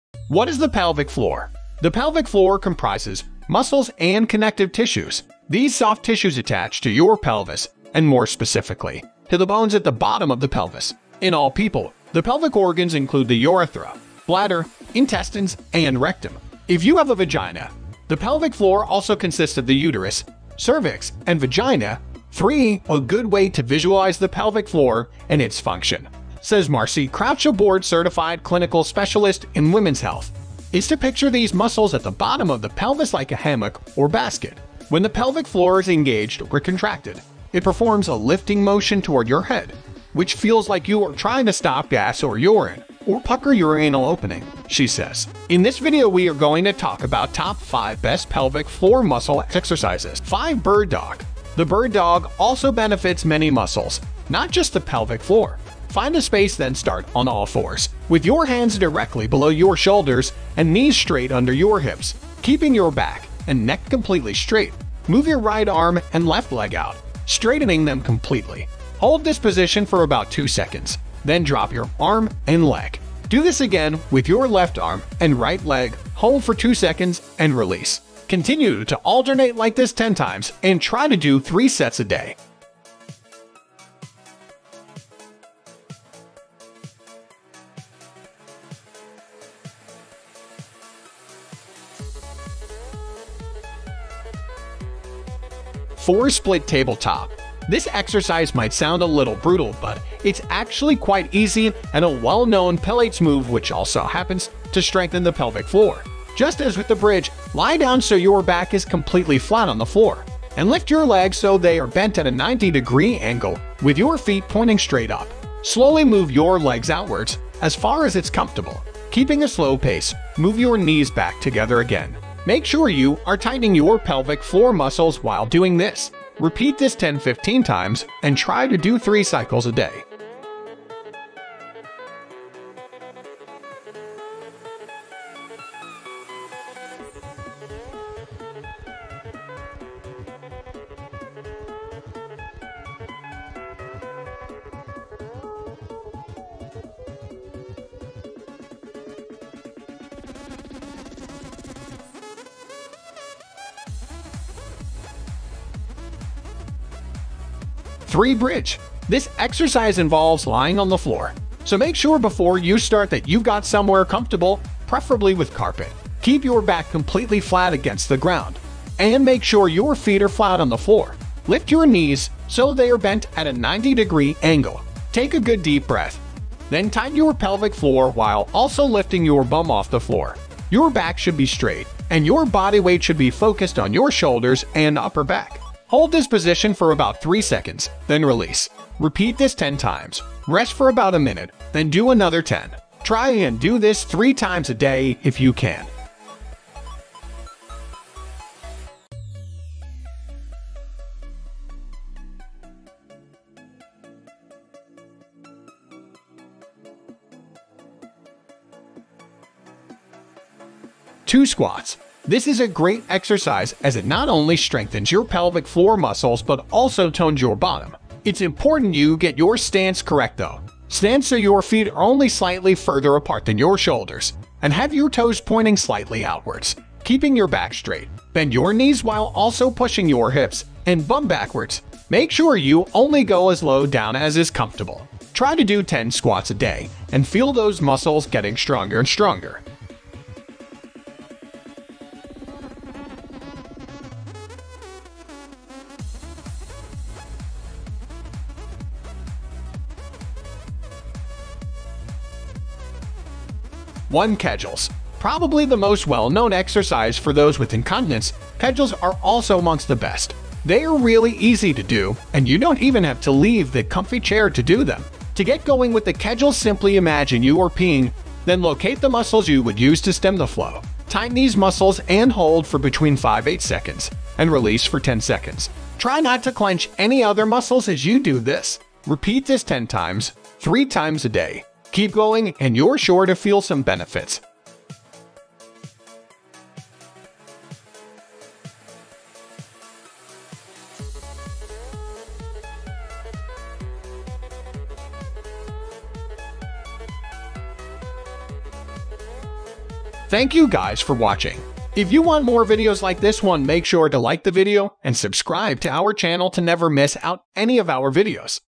These Are The TOP 5 Pelvic Floor Muscles Exercises For Man AND Woman (2022 Science)
Pelvic Floor Muscles Exercises For Men And Women - Anatomy and TOP 5 Exercises Explained. Follow-Along and start improving your pelvic health (Kegels).

Subscription is highly appreciated and helps us to produce more valuable content for you! 😀🙏

Other videos you’ll love:
This Happens When You Do 100 Push Ups Every Day | Doing 100 Push Ups Daily Transformation